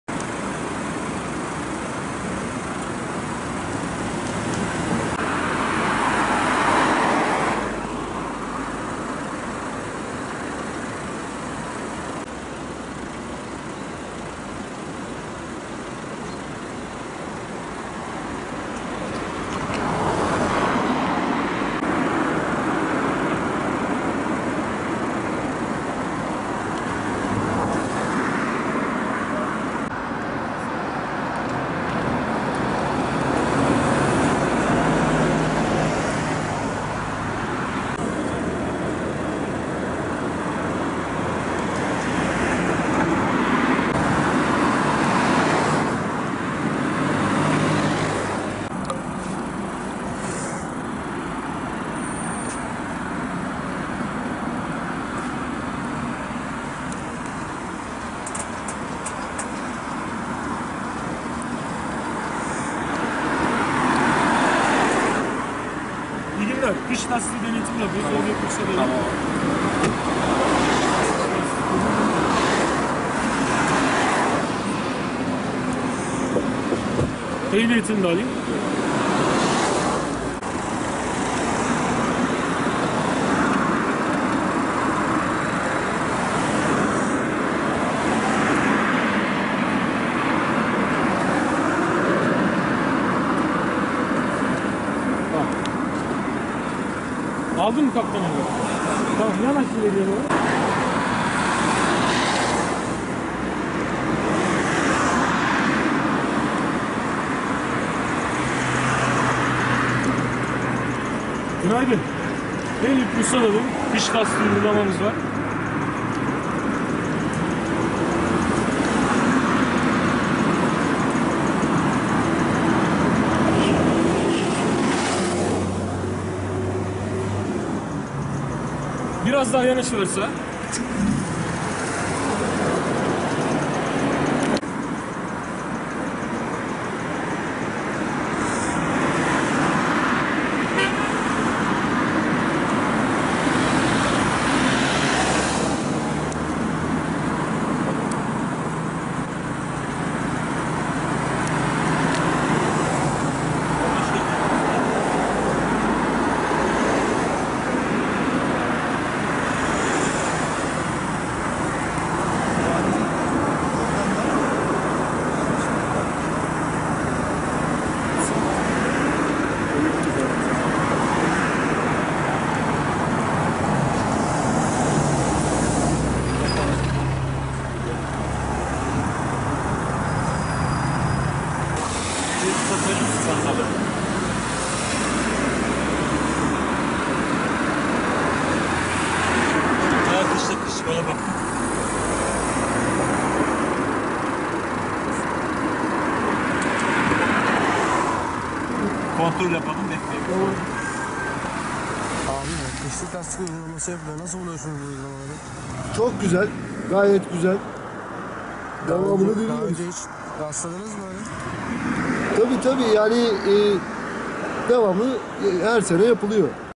Bursa'da kar lastiği olmayanlar yandı
Bursa'da, kış lastiği mecburiyetinin başlaması ile birlikte Emniyet Müdürlüğü Trafik Denetleme Şube Müdürlüğü ekipleri, sabahın erken saatlerinde kontrollere başladı.